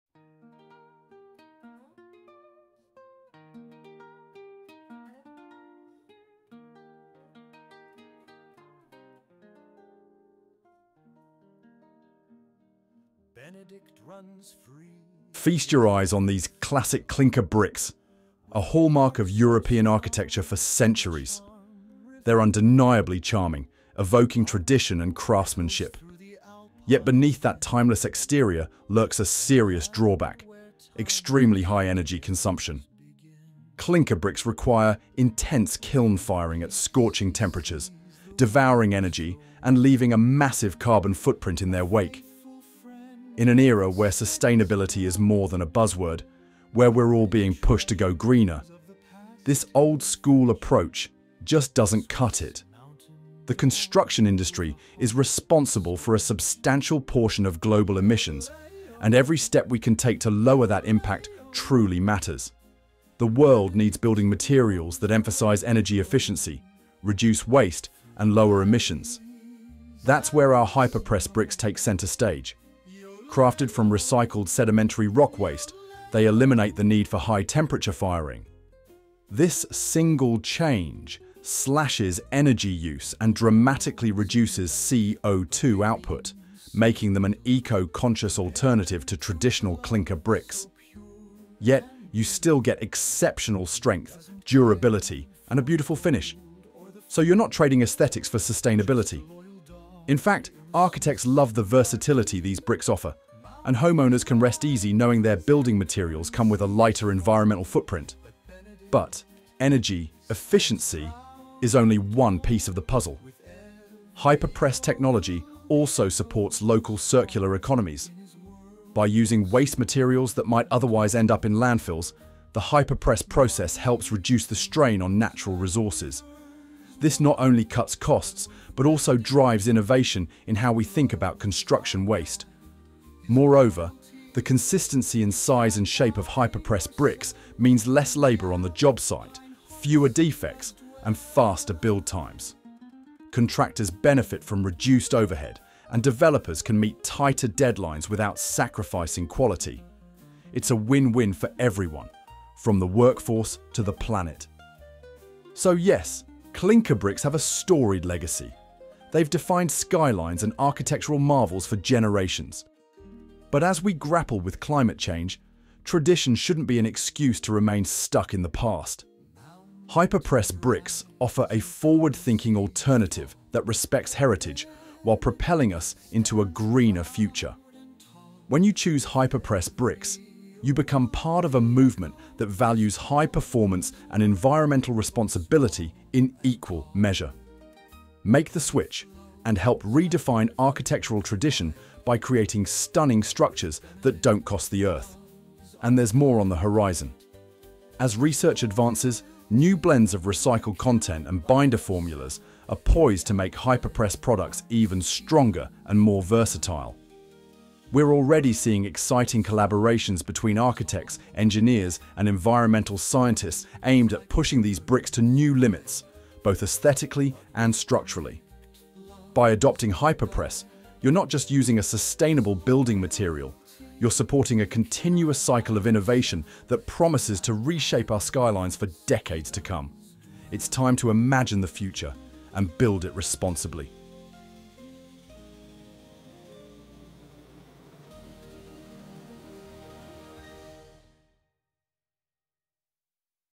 The Rise of Hyperpress Building a Greener Future
Feast your eyes on these classic clinker bricks—a hallmark of European architecture for centuries. They’re undeniably charming, evoking tradition and craftsmanship. Yet beneath that timeless exterior lurks a serious drawback: extremely high energy consumption. Clinker bricks require intense kiln firing at scorching temperatures, devouring energy and leaving a massive carbon footprint in their wake.
In an era where sustainability is more than a buzzword—where we’re all being pushed to go greener—this old-school approach just doesn’t cut it. The construction industry is responsible for a substantial portion of global emissions, and every step we can take to lower that impact truly matters. The world needs building materials that emphasize energy efficiency, reduce waste, and lower emissions. That’s where our hyperpress bricks take center stage.
Crafted from recycled sedimentary rock waste, they eliminate the need for high-temperature firing. This single change slashes energy use and dramatically reduces CO₂ output, making them an eco-conscious alternative to traditional clinker bricks. Yet, you still get exceptional strength, durability, and a beautiful finish—so you’re not trading aesthetics for sustainability. In fact, architects love the versatility these bricks offer, and homeowners can rest easy knowing their building materials come with a lighter environmental footprint.
But energy efficiency is only one piece of the puzzle. Hyperpress technology also supports local circular economies. By using waste materials that might otherwise end up in landfills, the hyperpress process helps reduce the strain on natural resources. This not only cuts costs but also drives innovation in how we think about construction waste.
Moreover, the consistency in size and shape of hyperpress bricks means less labor on the job site, fewer defects, and faster build times. Contractors benefit from reduced overhead, and developers can meet tighter deadlines without sacrificing quality. It’s a win-win for everyone—from the workforce to the planet.
So yes, clinker bricks have a storied legacy. They’ve defined skylines and architectural marvels for generations. But as we grapple with climate change, tradition shouldn’t be an excuse to remain stuck in the past. Hyperpress bricks offer a forward-thinking alternative that respects heritage while propelling us into a greener future.
When you choose hyperpress bricks, you become part of a movement that values high performance and environmental responsibility in equal measure. Make the switch—and help redefine architectural tradition by creating stunning structures that don’t cost the Earth.